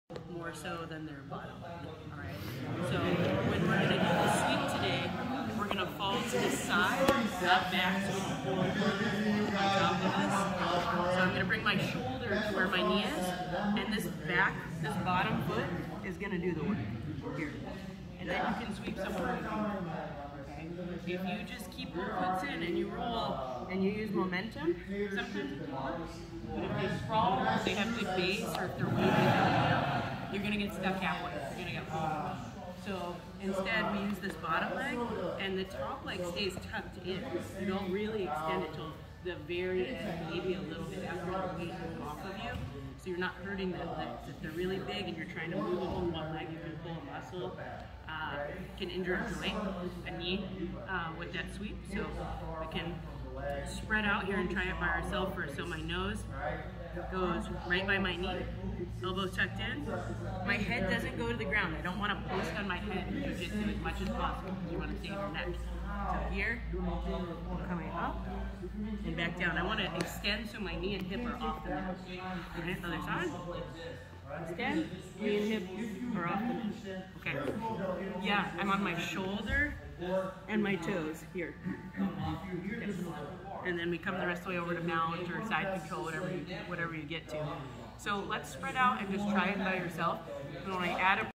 Leah Stumpf: Butterfly Guard Positioning and Movement
Leah Stumpf shares proper positioning and movement for butterfly guard at Origin Immersion Camp 2022.